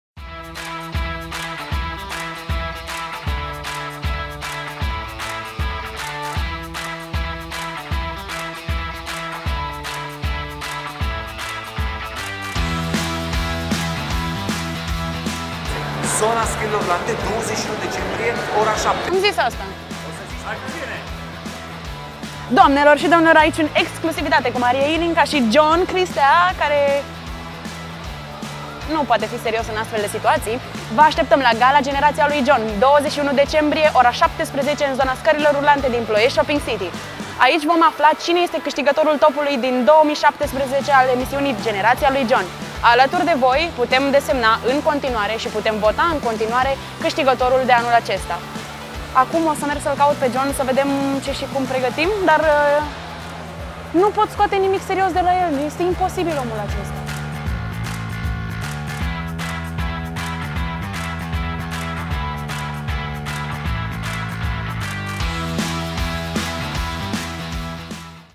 Promo Gala Generația lui John 2017
Vino la Gala Generația lui John, joi, 21 decembrie 2017, în Ploiești Shoppping City, zona scărilor rulante, ora 17:00.

Votează invitatul anului 2017: htttp://generatialuijohn.ro/top-21017